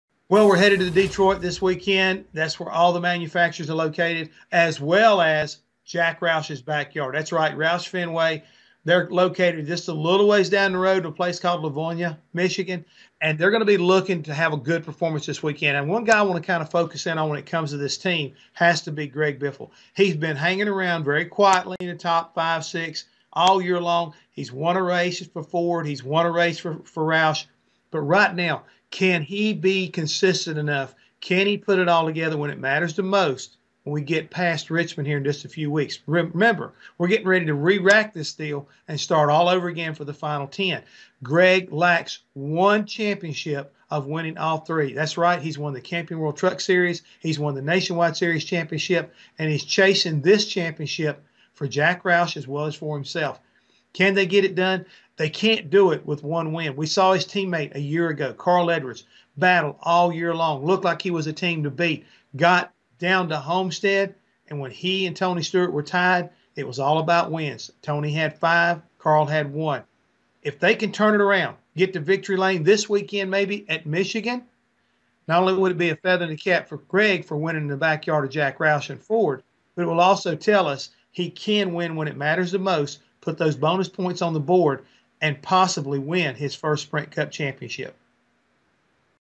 Hammond: Biffle needs wins
Does Greg Biffle have what it takes to win a championship? Jeff Hammond weighs in.